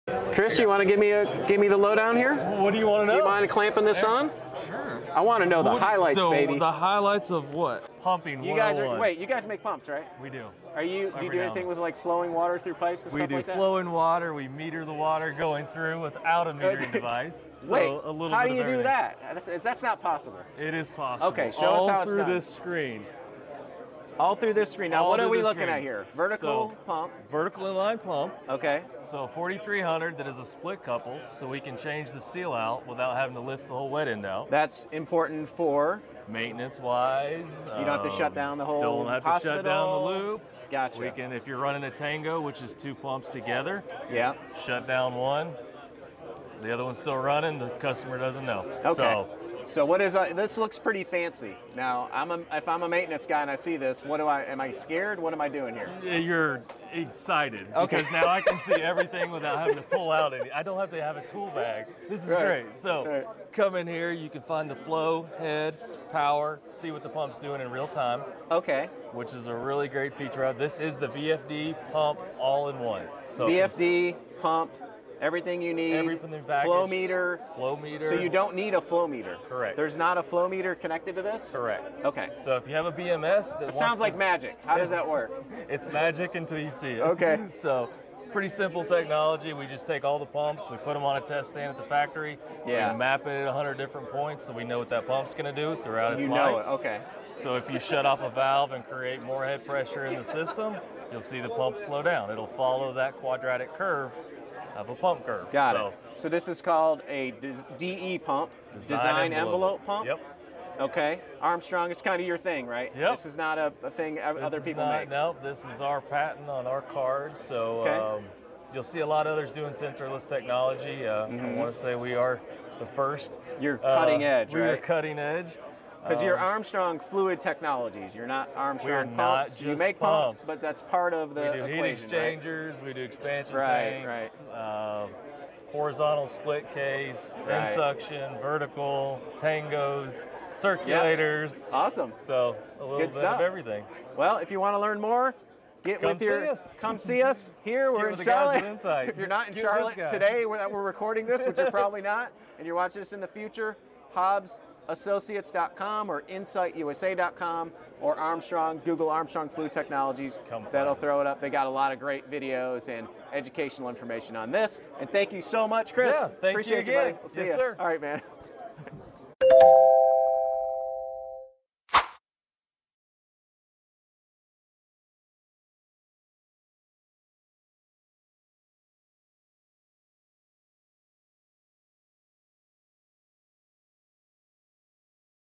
Understanding Design Envelope Vertical Inline Pumps | 3-Minute Introduction
Design Envelope technology is a demand-based intelligent control solution that models equipment and system behavior, monitors actual system conditions, and dynamically adjusts equipment operation to match system demand. Design Envelope technology is at the core of Armstrong’s groundbreaking advances in building performance. Design Envelope technology enables the greenest, most flexible, and most cost-effective fluid-flow and HVAC systems on the planet – resulting in both, lowest installed and lowest operating cost with the same equipment.